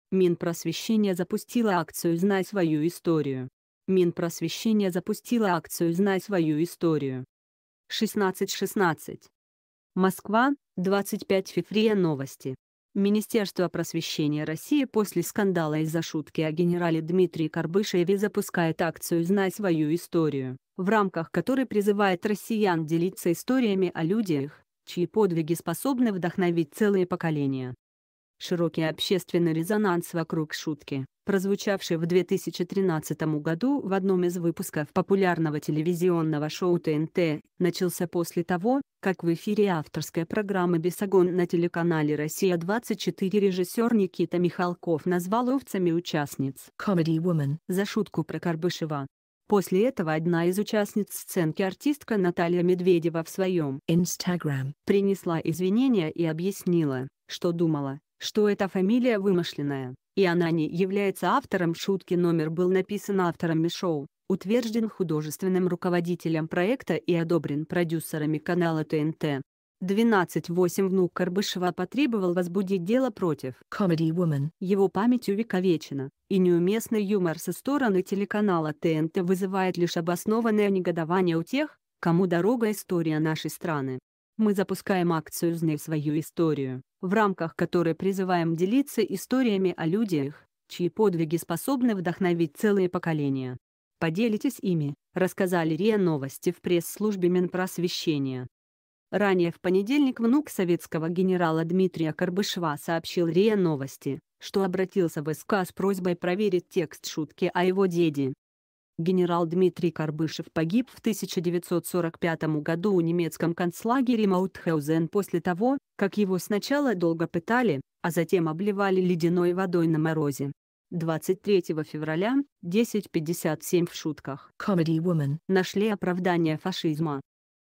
Минпросвещения запустило акцию Знай свою историю
Минпросвещения запустило акцию Знай свою историю Минпросвещения запустило акцию Знай свою историю 16 16 МОСКВА, 25 фев - РИА Новости . Министерство просвещения России после скандала из-за шутки о генерале Дмитрии Карбышеве запускает акцию Знай свою историю , в рамках которой призывает россиян делиться историями о людях, чьи подвиги способны вдохновить целые поколения . Широкий общественный резонанс вокруг шутки, прозвучавшей в 2013 году в одном из выпусков популярного телевизионного шоу ТНТ, началс